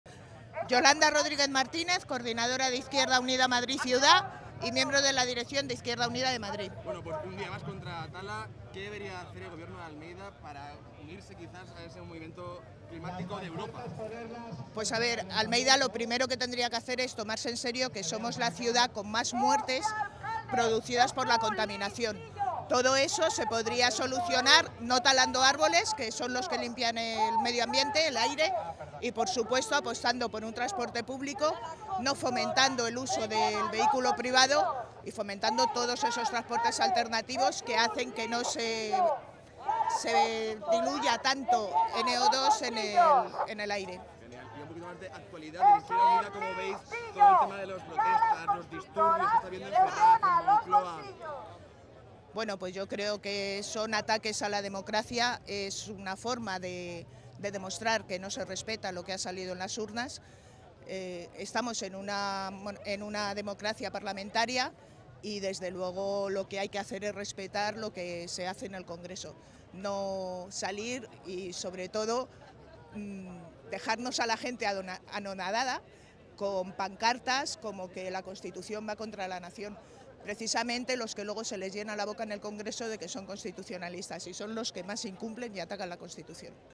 Izquierda Unida sobre los disturbios frente a la sede del PSOE: "Es un ataque a la democracia"
Yolanda Rodríguez, coordinadora de IU en Madrid, ha acudido a la movilización contra la tala frente al ayuntamiento de Madrid, donde ha asegurado que José Luis Martínez Almeida, alcalde de Madrid, debería "tomarse en serio" que Madrid es "la ciudad con más muertes provocadas por la contaminación". También ha valorado las movilizaciones y los disturbios que se han visto en Ferraz frente a la sede del Partido Socialista y frente al Palacio de la Moncloa, donde ha asegurado que es un "ataque a la democracia".